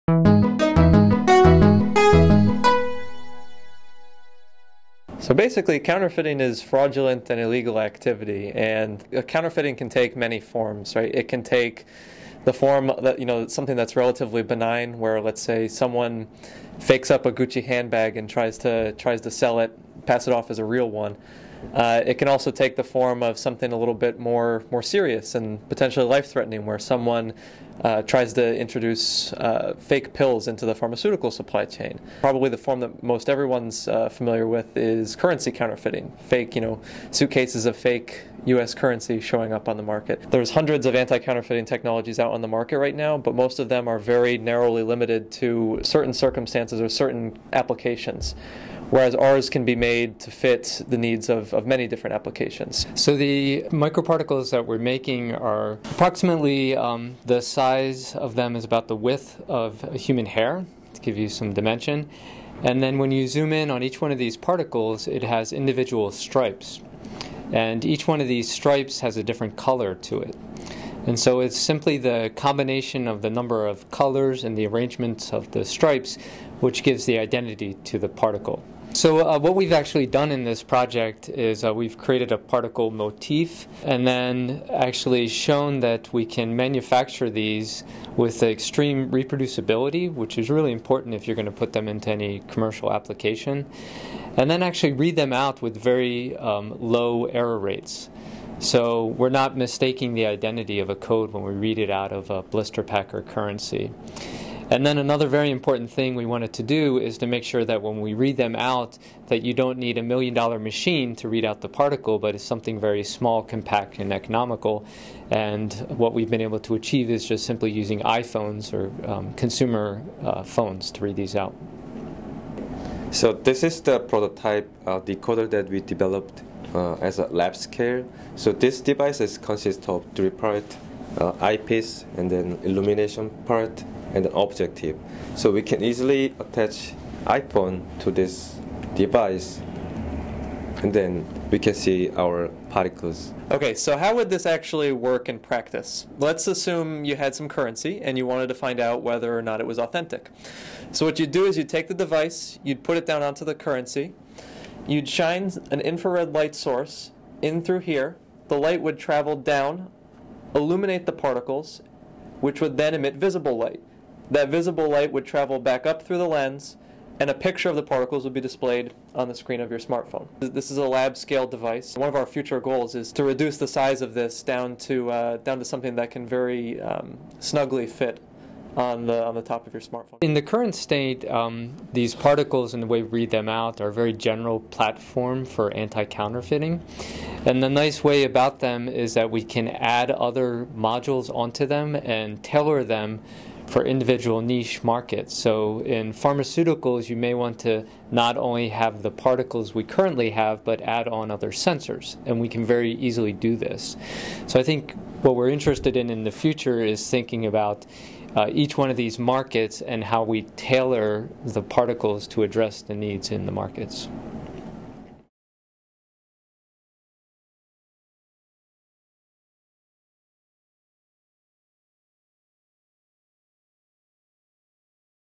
Smartphone-readable microparticles crack down on counterfeiting
Some 2 to 5 percent of all international trade involves counterfeit goods, according to a 2013 United Nations report. These illicit products — which include electronics, automotive and aircraft parts, pharmaceuticals, and food — can pose safety risks and cost governments and private companies hundreds of billions of dollars annually.

Many strategies have been developed to try to label legitimate products and prevent illegal trade — but these tags are often too easy to fake, are unreliable, or cost too much to implement, according to MIT researchers who have developed a new alternative.

Led by MIT chemical engineering professor Patrick Doyle and Lincoln Laboratory technical staff member Albert Swiston, the researchers have invented a new type of tiny, smartphone-readable particle that they believe could be deployed to help authenticate currency, electronic parts, and luxury goods, among other products. The particles, which are invisible to the naked eye, contain colored stripes of nanocrystals that glow brightly when lit up with near-infrared light.

Video: Melanie Gonick, MIT News
Supplemental materials courtesy of the researchers